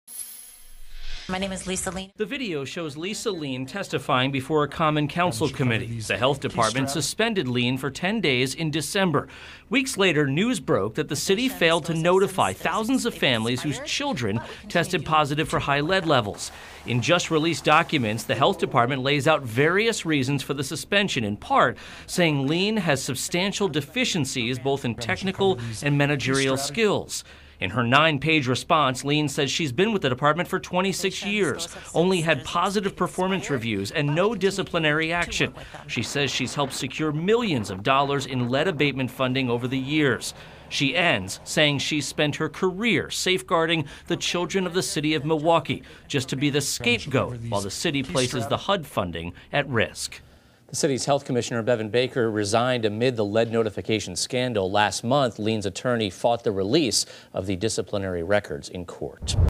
Documents shed new light on Milwaukee's Lead Abatement Program
In new documents released on Monday, WISN 12 News has learned more about the suspension of the person in charge of the Lead Abatement Program.